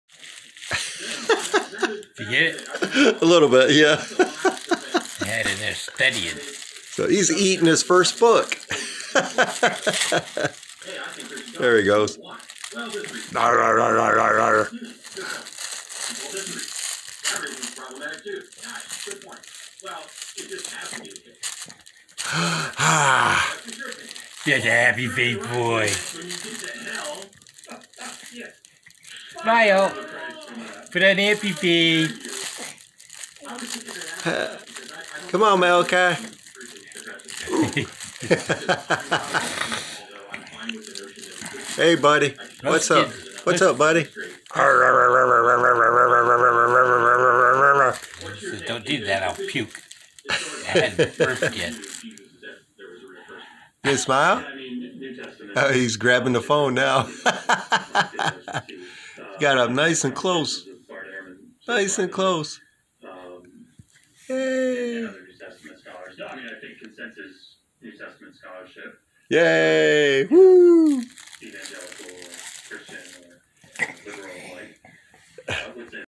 Eating his first book. lol
Playing with Gus and grandpa. Eating his first book.